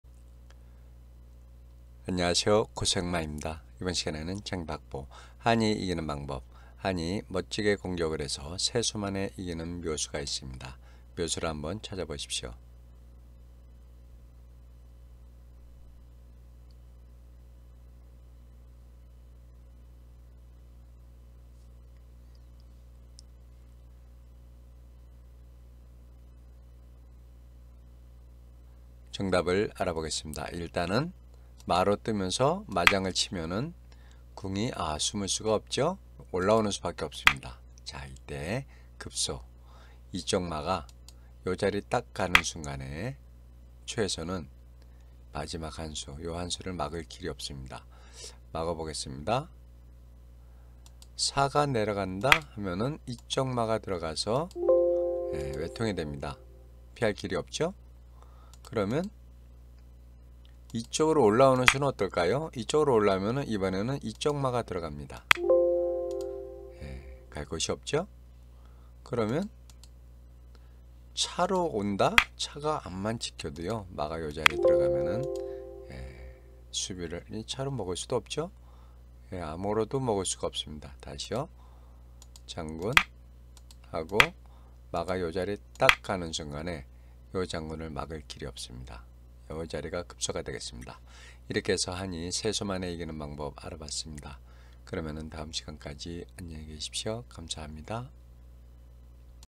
장기박보묘수 - 양마공격으로 3수만에 외통만들기
상대수비못하게 양마공격 - 장기박보묘수